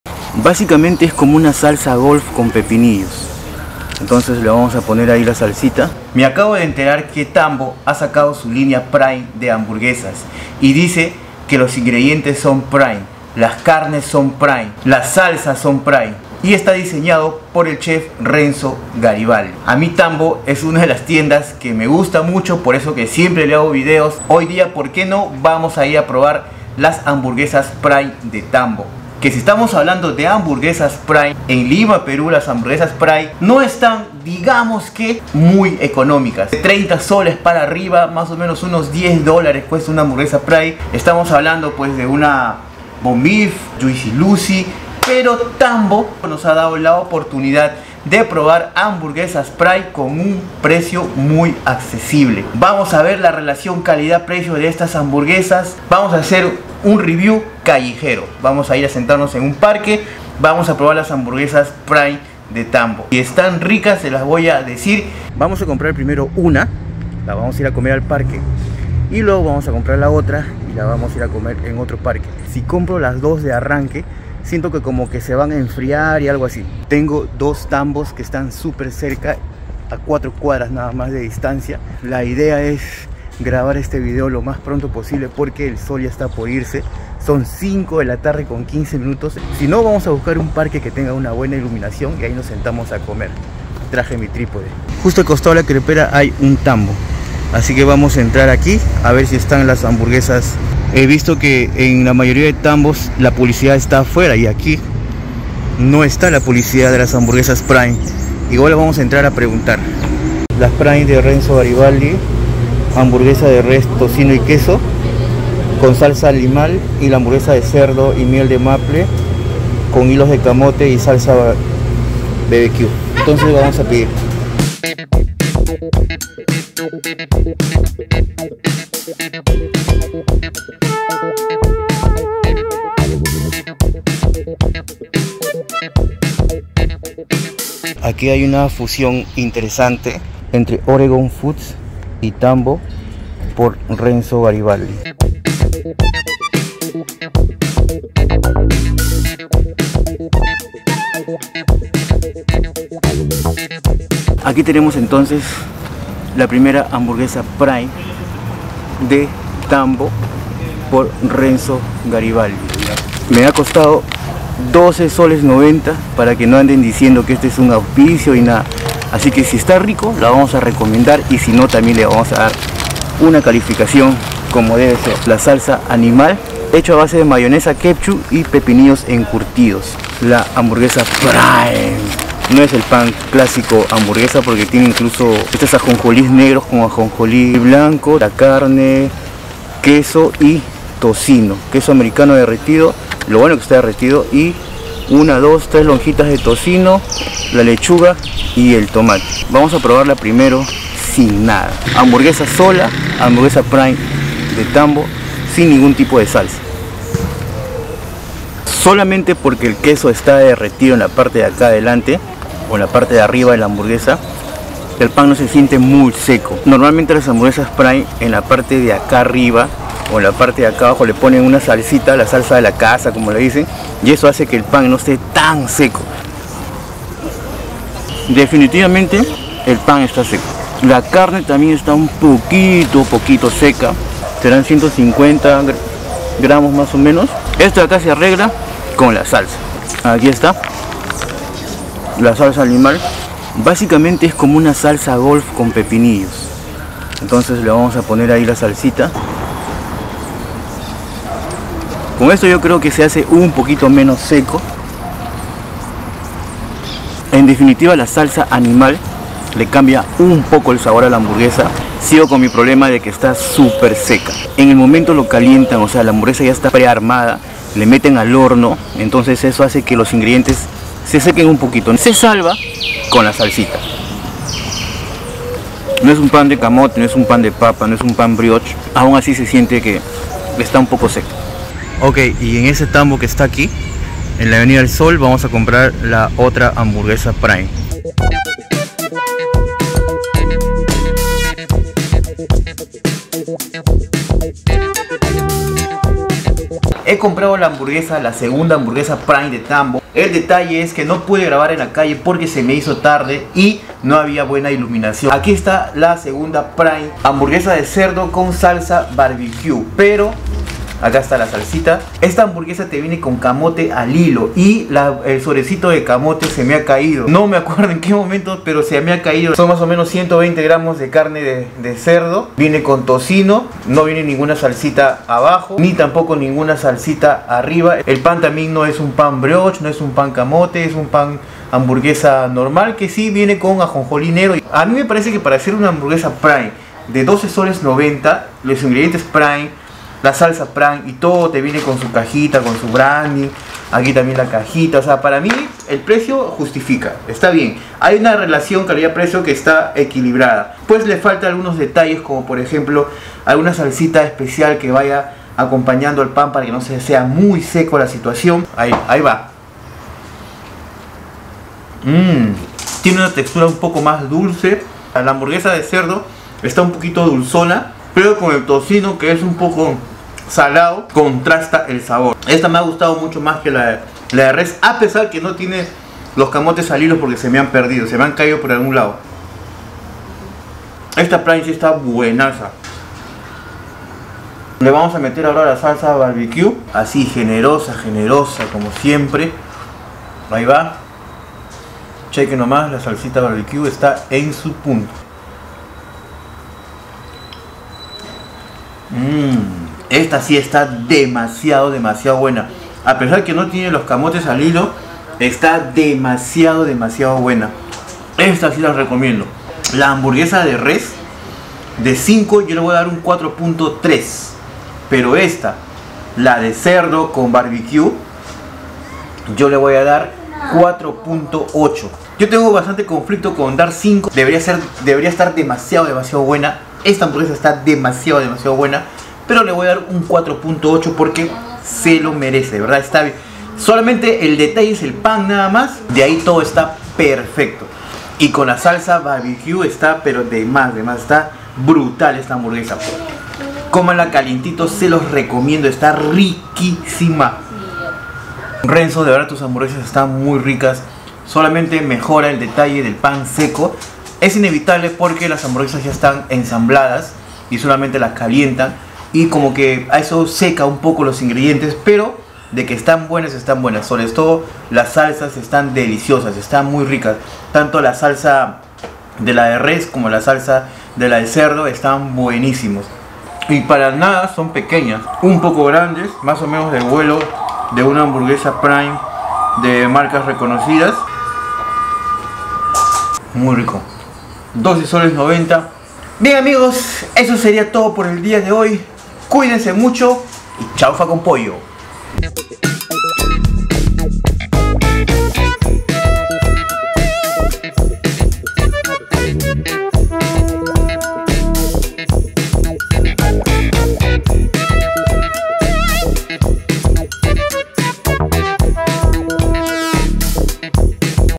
¿QUE TAL SON LAS HAMBURGUESAS PRIME DE TAMBO?
Salimos a probar las hamburguesas Prime de Tambo y están muy buenas; la relación calidad precio están en su punto.
No nos pongamos exigentes porque el precio esta muy bien y los ingredientes están buenos.